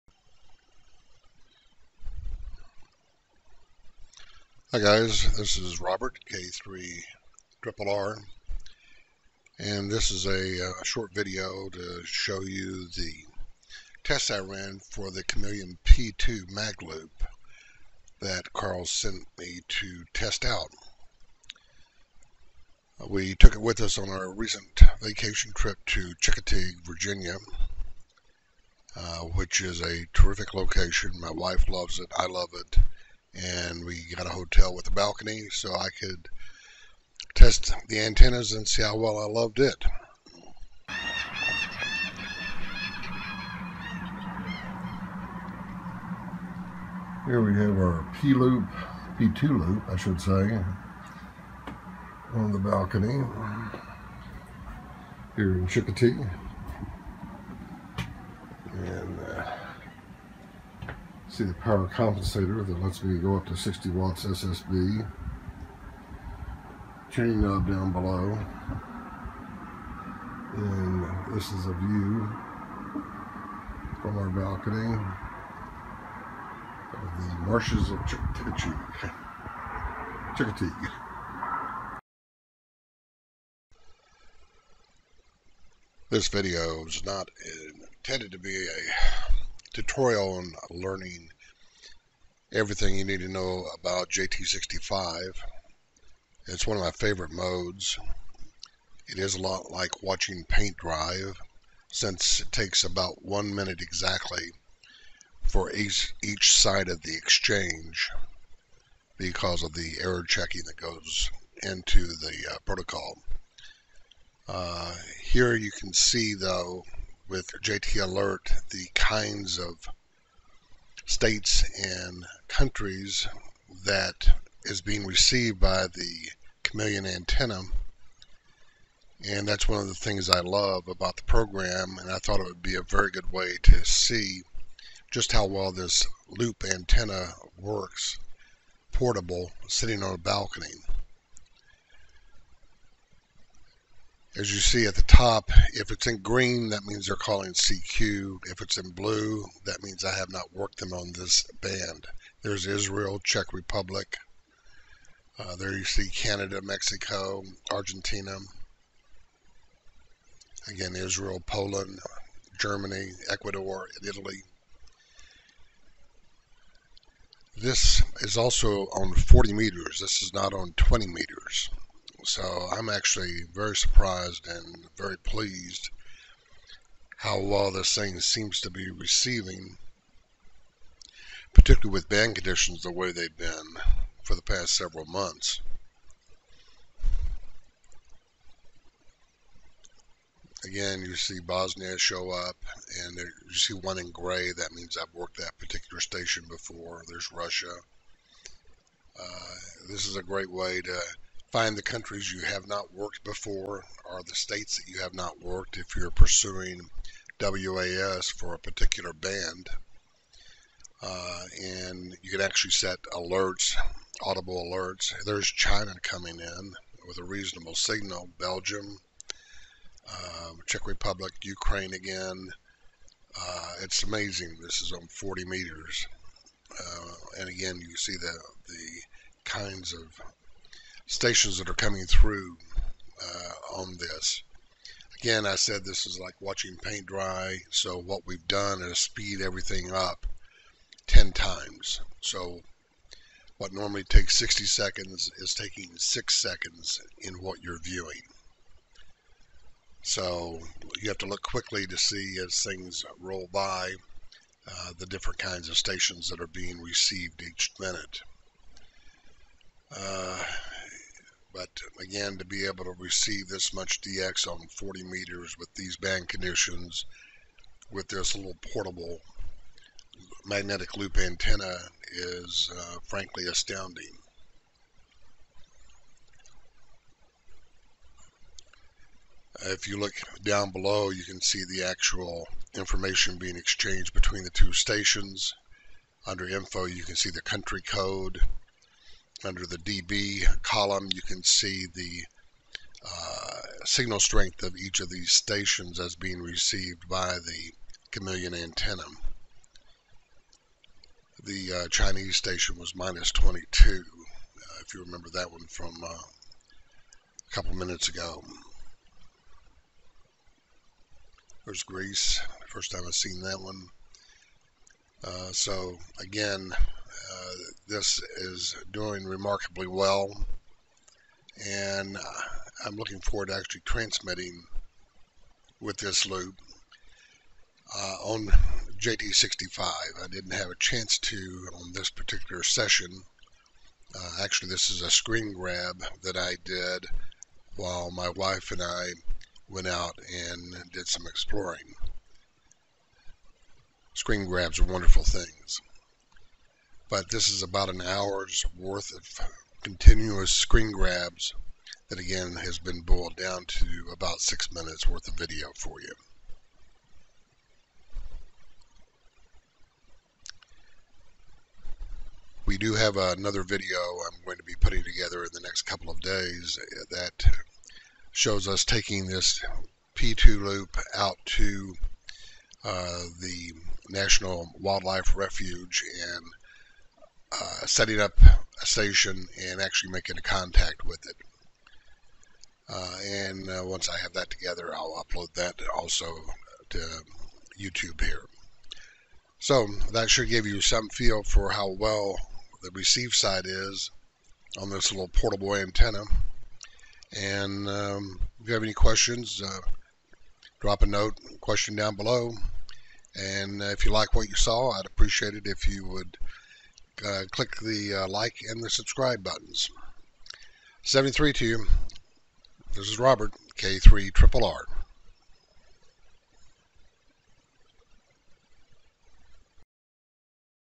Chameleon P-Loop 2 Mag Loop Antenna Receiving JT-65 HF HB9HQX 10X Timelapse
:::::::::::: More at K3RRR.com

This video was designed as a test of the new Chameleon P-Loop 2 Mag Loop Antenna  that the company just released.

Carl at Chameleon sent me one to run through the ringer and test out so we can see how well it works in our typical locations for portable operations.

Fortunately, it arrived just in time for us to take it with us on our recent vacation to Chincoteague Island Virginia located very near the Atlantic Ocean and about an hour's drive south of the Maryland / Virginia border.

My wife and I both love the National Wildlife Reserve on Chincoteague which is why we continue to choose it for vacations.

I specifically chose a hotel with a balcony for this trip – to both enjoy the view and also test our antennas. You will see in the videos that I have two antennas on our balcony: the Chameleon P-Loop 2 Mag Loop Antenna long with a dual band log periodic 2 m/440 MHz ELK antenna that I use for working the amateur radio satellites.

By the way, the transceiver was a Yaesu FT 991.

Besides being a fan of ham radio satellites and portable operations, I am also a big fan of the JT-65 digital mode. My favorite version of JT-65 HF software is the one developed by HB9HQX.

One of the things that makes JT-65 so enjoyable for me is a free add-on program called JT Alert developed by Laurie VK3AMA. The JT Alert has a multitude of features and options but the key thing I like best is that you can easily see the state or country of the stations coming through the waterfall. You can also have it alert you when a station in a country or state that you need for some award happens to also come to that same waterfall.

However, the reason I chose JT Alert and JT-65 to demonstrate the receive capability of the Chameleon P-Loop 2 Mag Loop Antenna  was that it made it very easy for the viewer to see the different stations and where they are located - without requiring them to understand the Zen of JT-65.

The bad news about JT-65 is that it is like watching paint dry since it takes exactly 1 minute for just one side of each QSO. Fortunately, through the magic of Camtasia, I was able to speed this up and turn the screen grab into a 10X time lapse so that each 60 second cycle time only took six seconds as you view the video.

One of the key things to remember is that the Chameleon Loop is receiving 40 m and not 20 m when you see the multitude of DX stations that show up on the waterfall – including China!

In summary, I am genuinely amazed at the 40 m DX the small magnetic loop was able to receive – especially taking into account the abysmal band conditions we had for the past several months!

I wonder which viewer will first tell me the correct number of countries and the correct number of states that show up in this one hour test!

73 de Robert K3RRR
K3RRR.com

PS: Here are some useful links:

Chameleon P Loop 2.0

Chameleon F Loop Plus